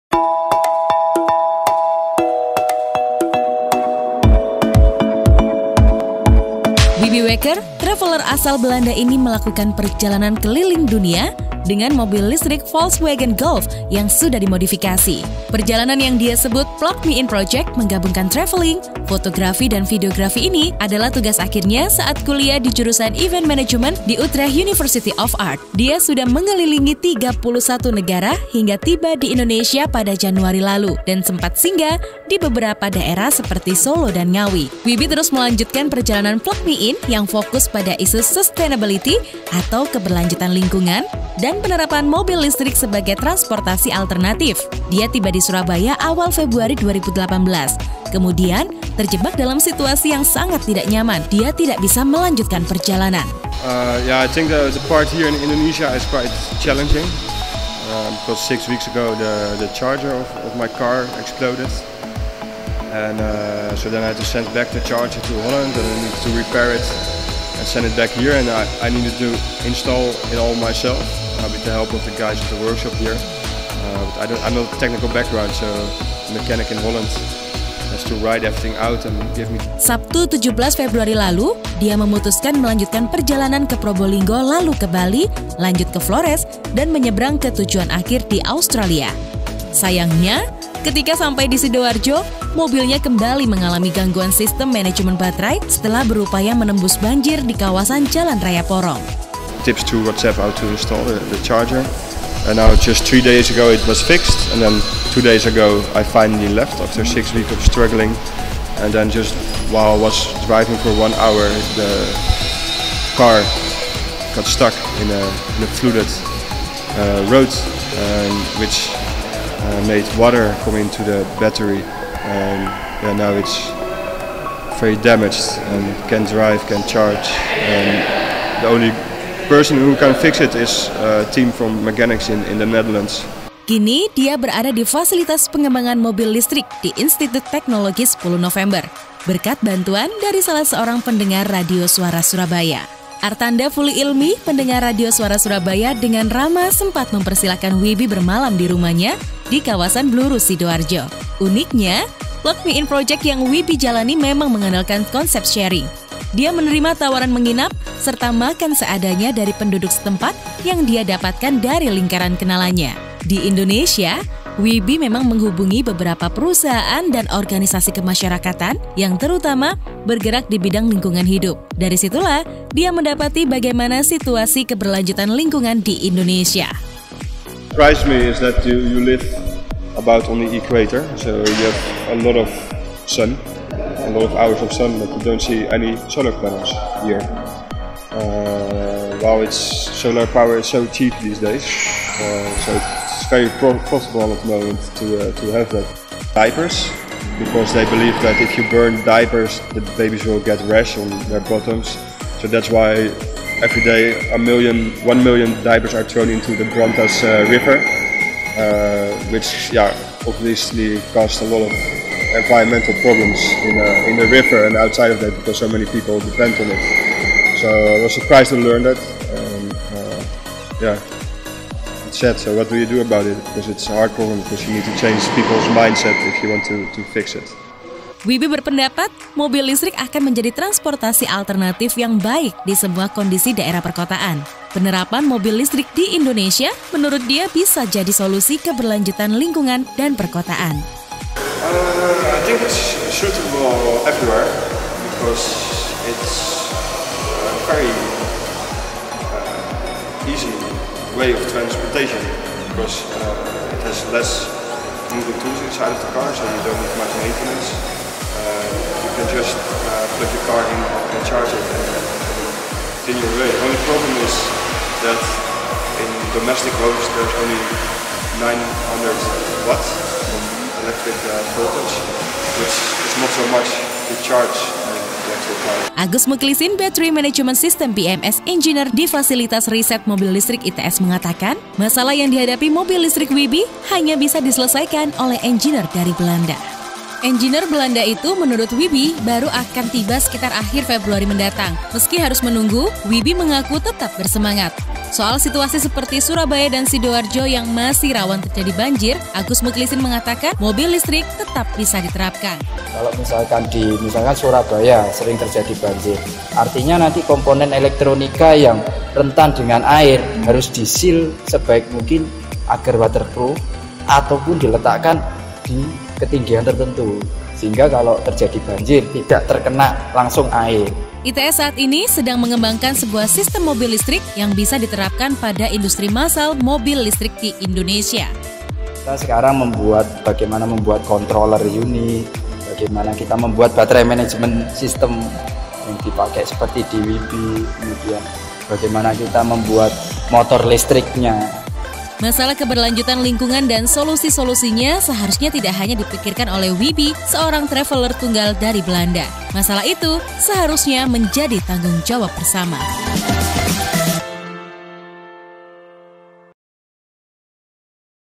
Wiebe Wakker, Traveller Belanda yang Keliling Dunia Dengan Mobil Listrik
Wiebe Wakker, Traveller Belanda yang Keliling Dunia Dengan Mobil Listrik. Dia tiba di Surabaya awal Februari yang lalu. Namun ada kendala yang ia hadapi yakni baterai mobilnya mengalami gangguan setelah melewati banjir di kawasan Jalan Raya Porong. Saat ini Dia berada di ITS, menunggu teknisi mobil dari Belanda untuk memperbaiki mobilnya.